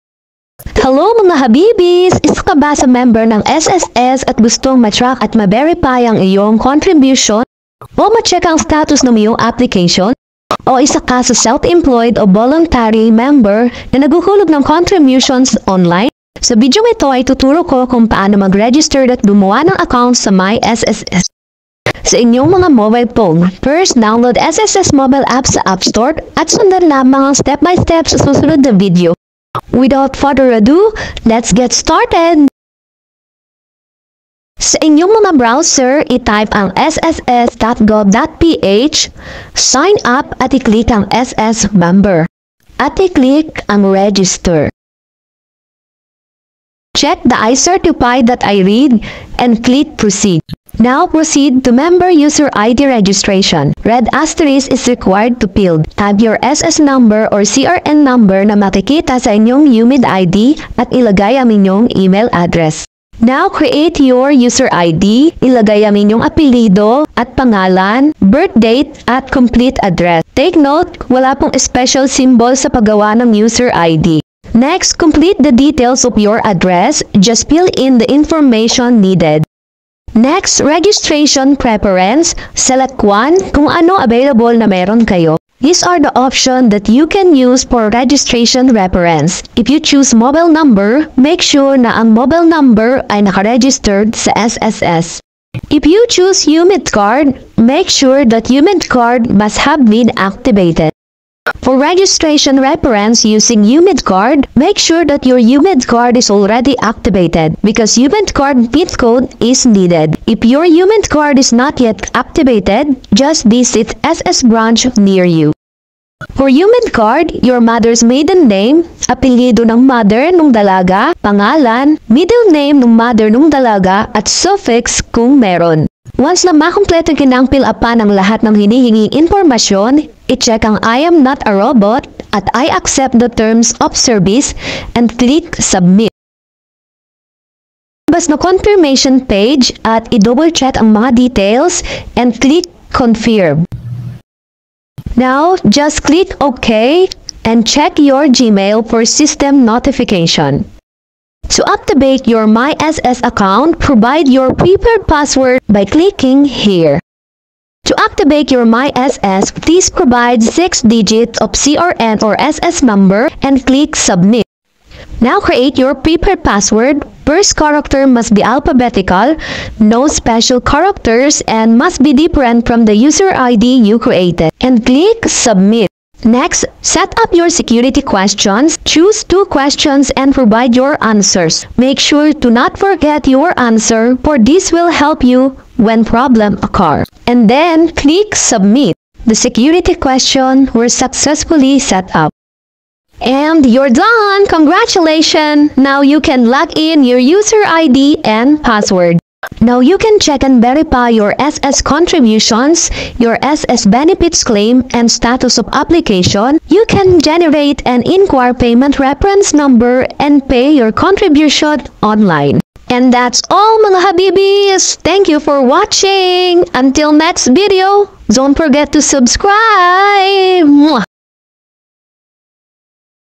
PAANO MAG REGISTER AT GUMAWA NG ACCOUNT sa MY.SS step by step tutorials/2024
hello mga habiBees
another tutorials kung paano
gumawa ng account sa my.SS
madali mo nang ma aAccess ang itong contribution at iyong application status,
and benefits claim.

sundan lamang ang steps at tapusin ang video, mangyari i comment kung may karagdagang tanong.

if you think this video helps